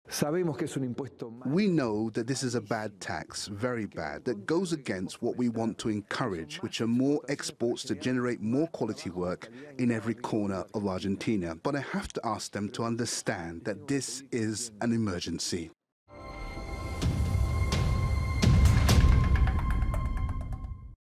Macri pleads for understanding as Argentina turns to austerity
Argentina's president Mauricio Macri says he will provide more aid and food programmes for Argentinians hit by soaring inflation.